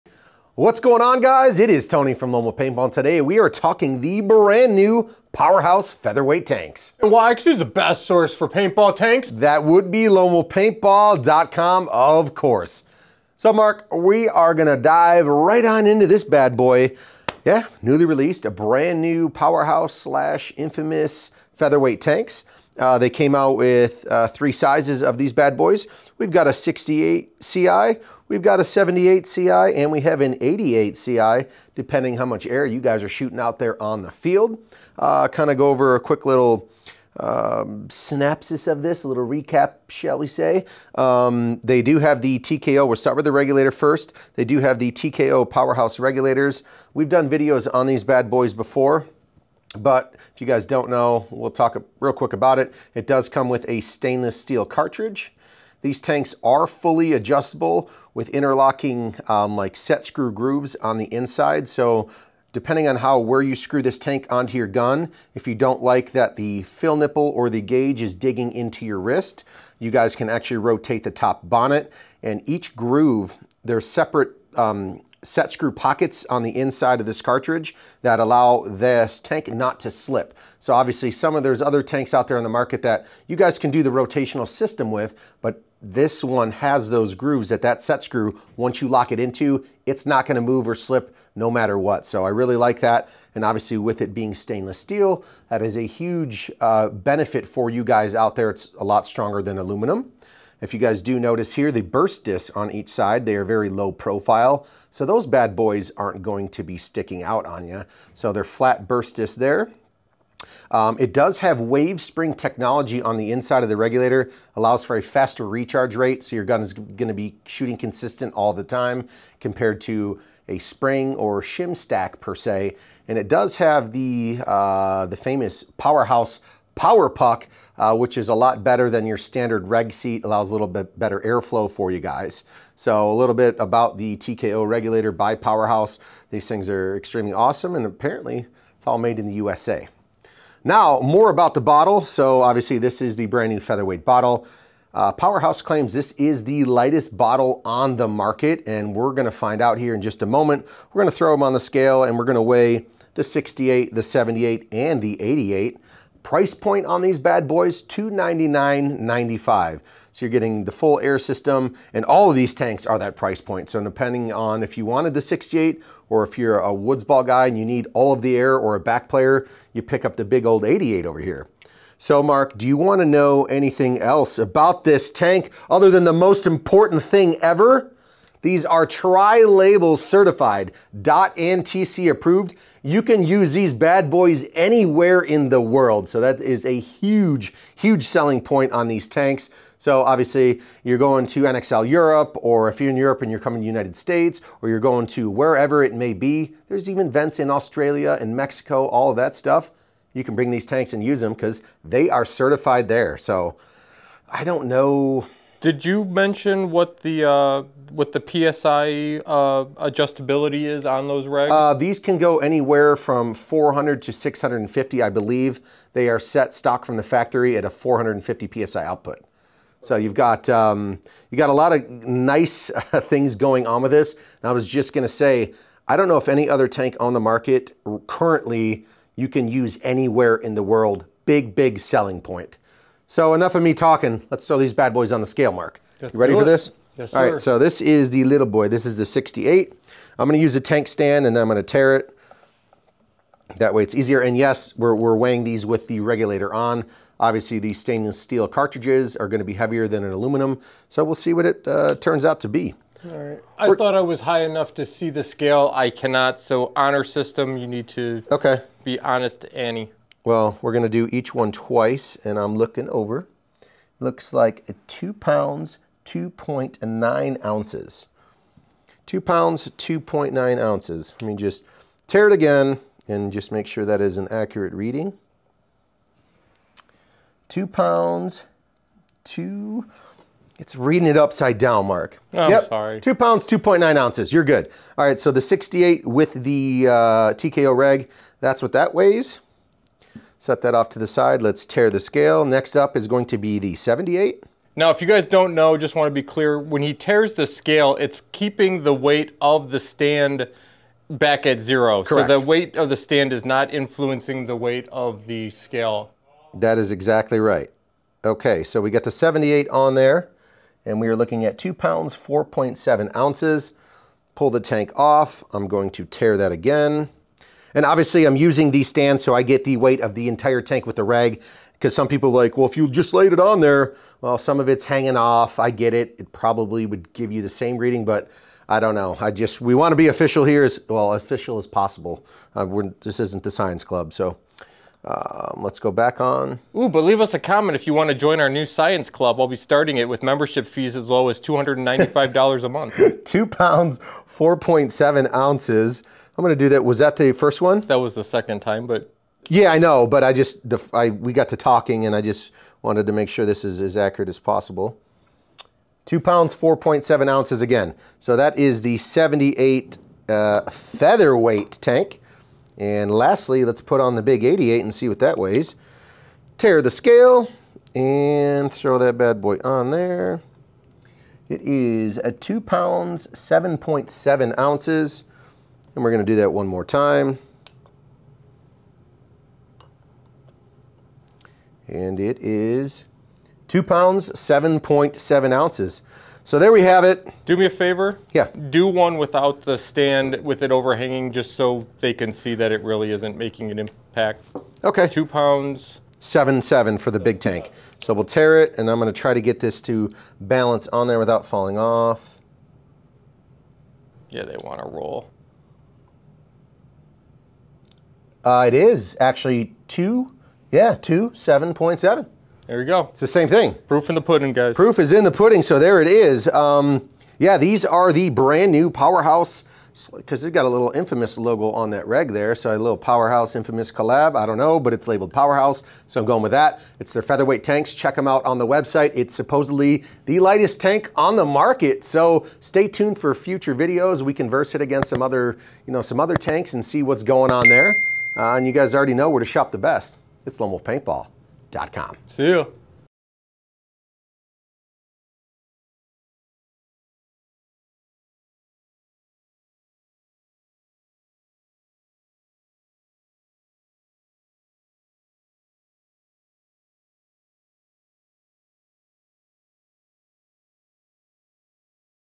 Powerhouse Featherweight Air System | The Lightest Air Tanks? | Lone Wolf
Powerhouse Featherweight Air System | The Lightest Air Tanks?
Order the Powerhouse Featherweight Air System from the best source for paintball

Powerhouse™ proudly introduces their line of “Featherweight” Air Cylinders.  The featherweights are the lightest paintball tanks available on the market today.  All Powerhouse™ tanks feature Tri-Label UNISO certification. This is the highest standard in paintball and covers both DOT and TC certification. These certifications allow these cylinders to be used anywhere in the world.

Available in 3 convenient sizes: 68ci, 78ci, and 88ci you can now match the size and amount of air needed to suit your style of play. On those long layout points slap on your 88, but on the quick points go for the lightweight 68. Not sure, well the 78 is the perfect mix.

Use these state of the art Featherweight tanks with the LATEST Gen Powerhouse™ TKO Regulator and you have the absolute best available air system in paintball.

The Powerhouse™ Featherweight Air System provides the ideal combination of the lightest weight carbon fiber tank with the highest performance regulator on the market today.  The pinnacle of air systems.